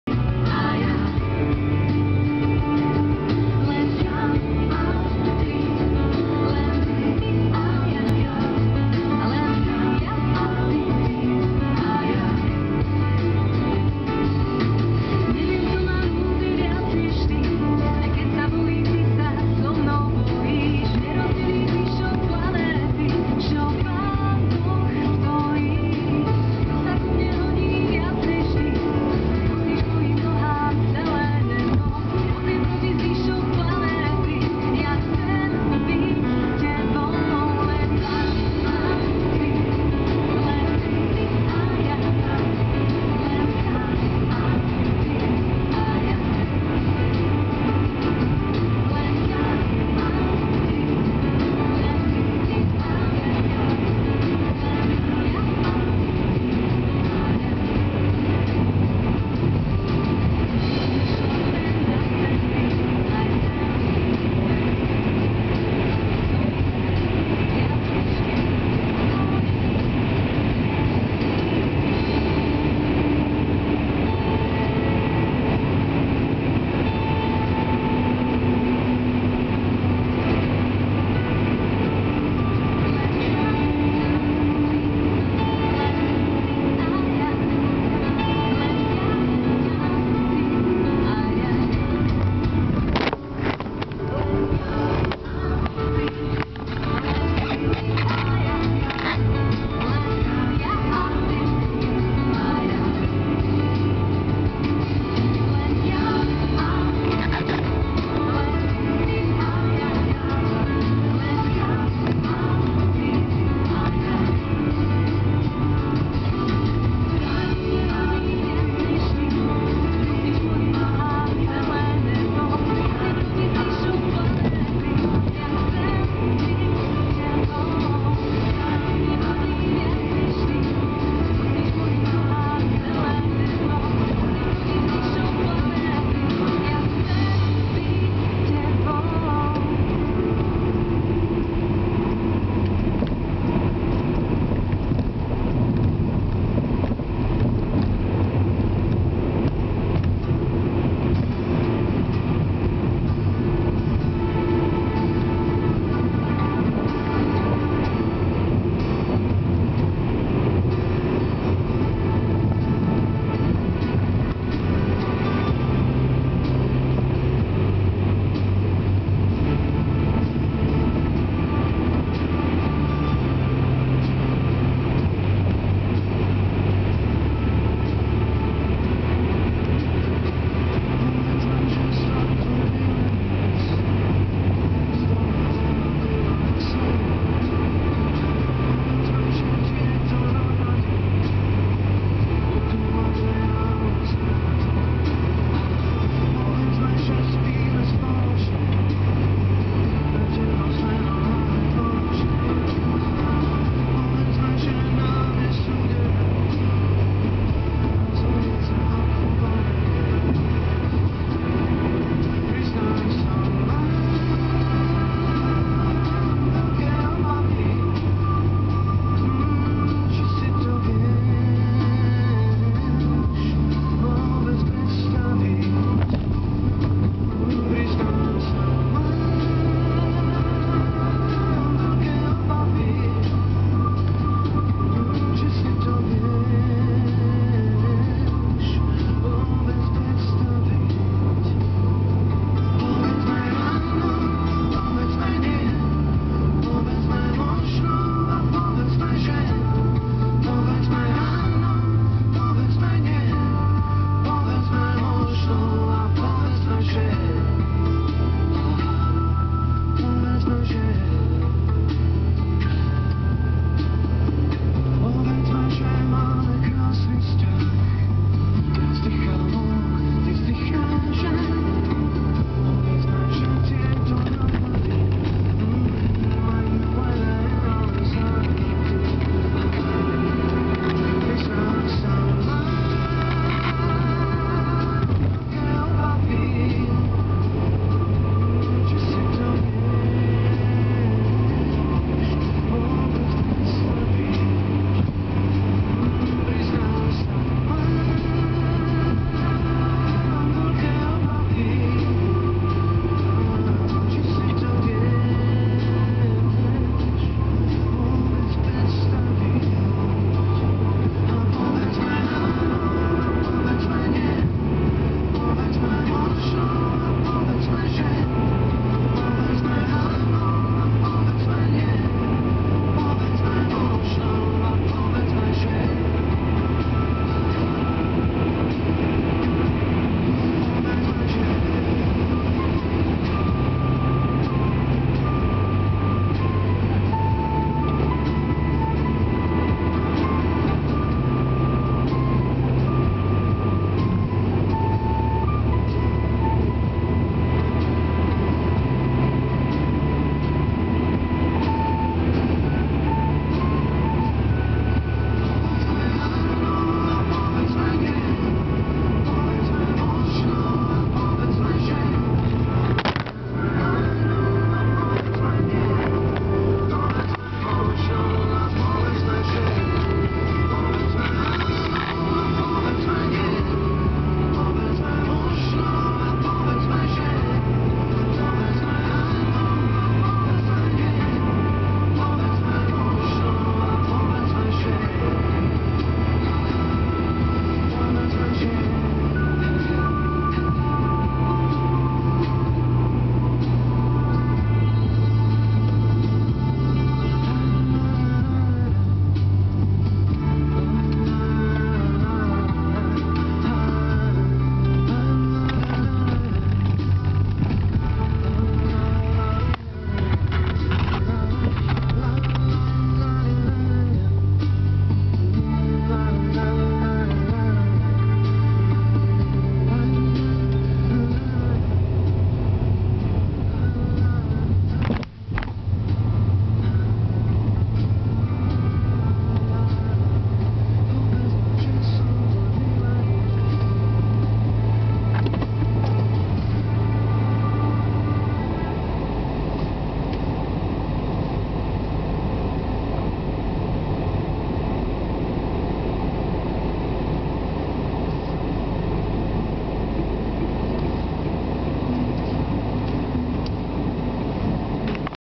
Drive from Lokca via Namestovo to Slanicka Osada
Early in the morning from Lokca (using the Orava Highway) passing Tapesovo to Namestovo. There crossing the Orava to Slanicka Osada. (Sorry that the cam dropped twice. Really bad roads)